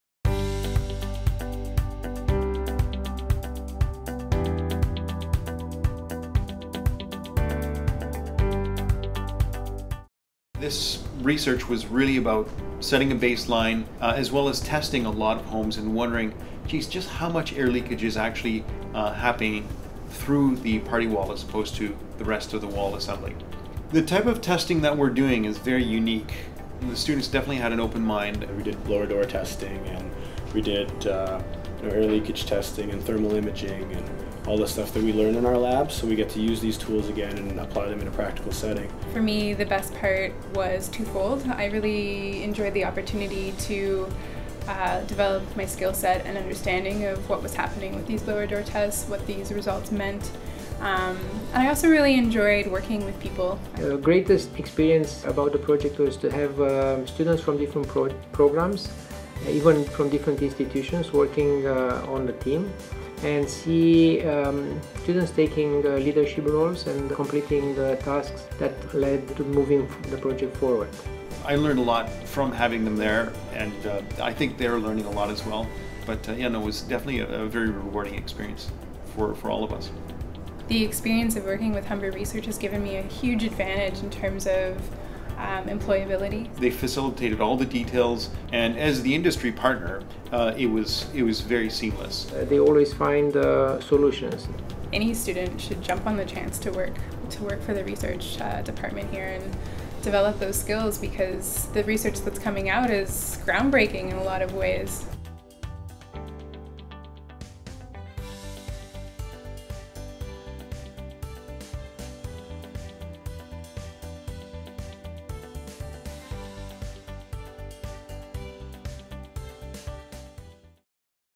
Research Collaboration - Humber Research and BlueGreen Consulting Group
Humber faculty Dr. Dragos Paraschiv and Greg Labbé, President, BlueGreen Consulting Group, discuss their NSERC funded project "Common Wall Air Leakage Quantification in Solid Masonry Semi-detached Homes."